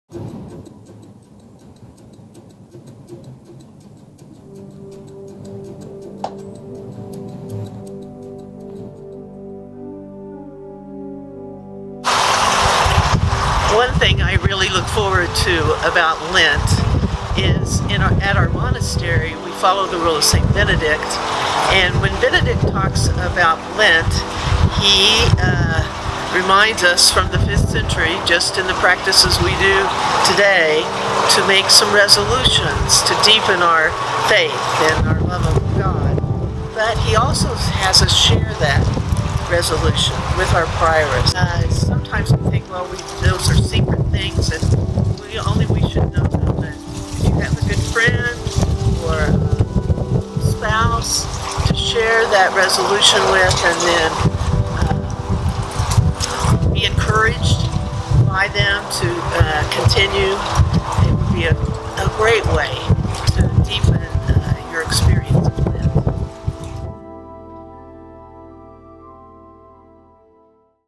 40 Days in 90 Seconds: Sister Barbara Austin OSB
40 Days of Lent explained by a Catholic we all know in 90 Seconds